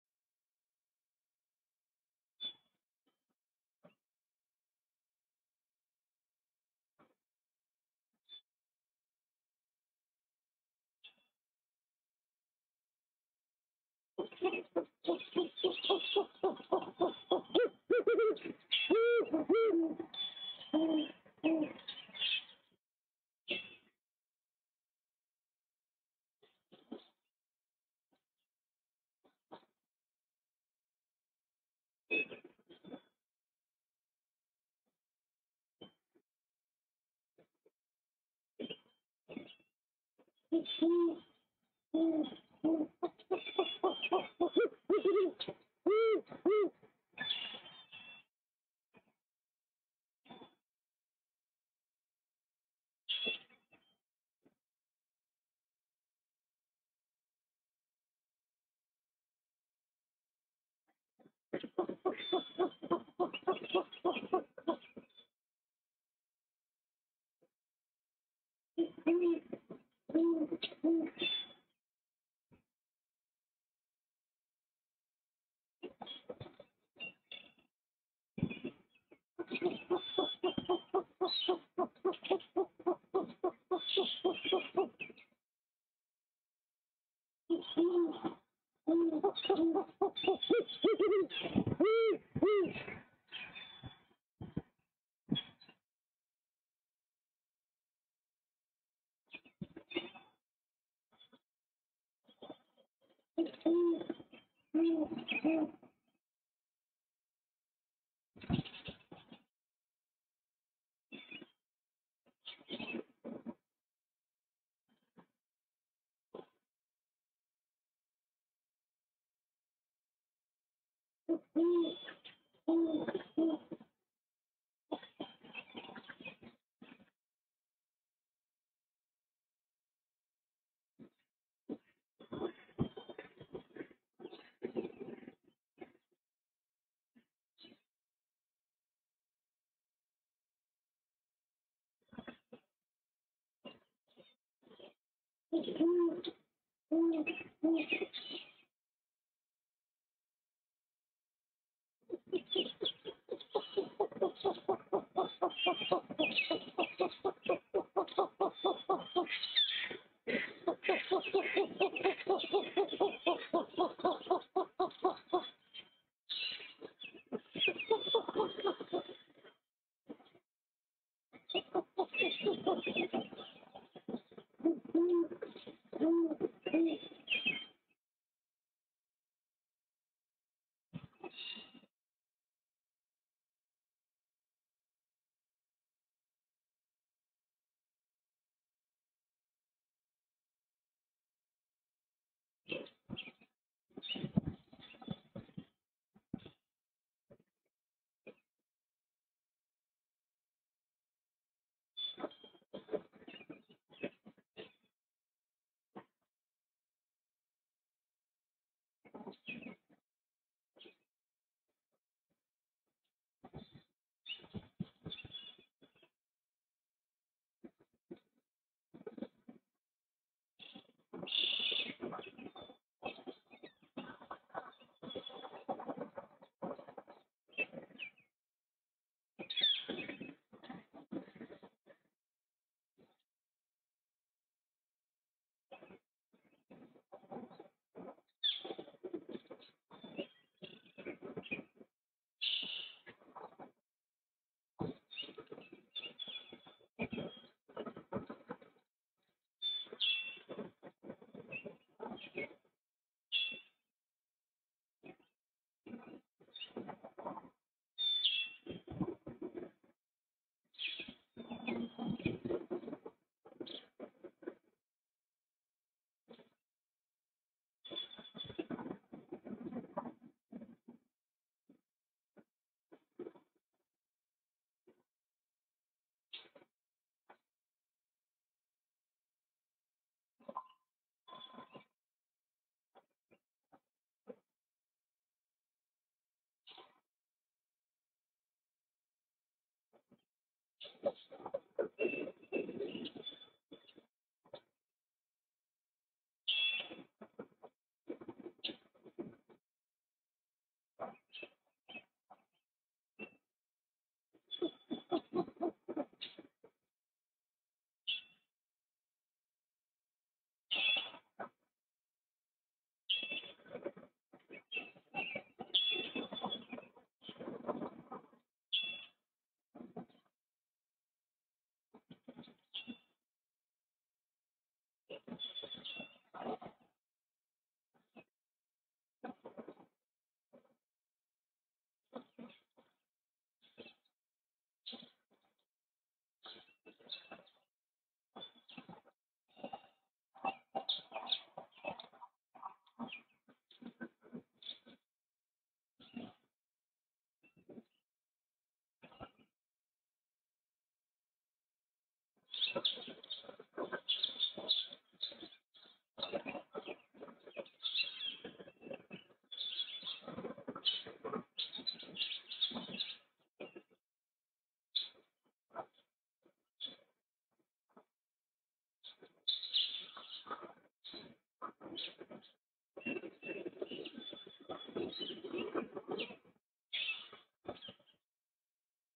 1st Owlet is one month old - Ellis Bird Farm GHO Nest
Dates the Eggs were Laid: March 4th, 8th, and 12th.
1st Egg: hatch confirmed 04:30 am April 7th
2nd Egg: hatch confirmed 04:15 am April 10th
3rd Egg: hatch confirmed April 14th hatch likely took place the day before.